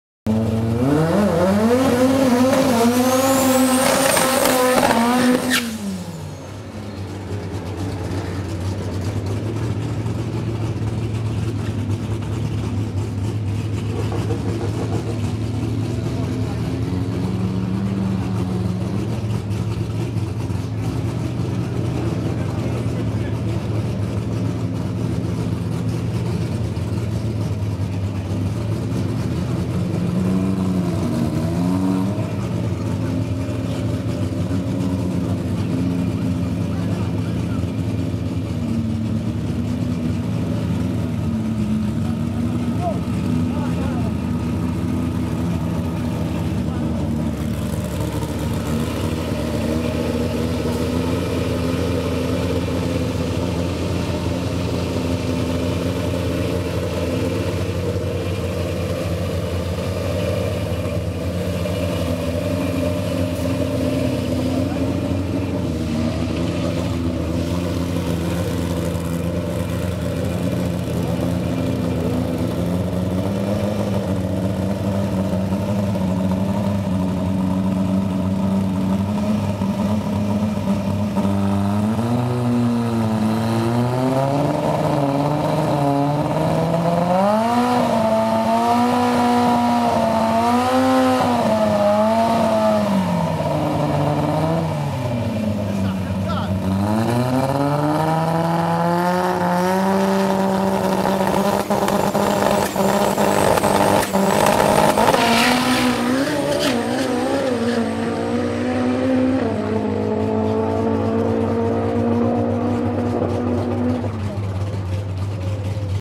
November 2012 QTM Day 1 Run 100
23rd November 2012 - Day 1 of the Final QTM event for 2012, held at Hal Far Raceway... Today was the Practice day, ie Test and Tune day, before the actual qualification runs which start tomorrow, 24th November 2012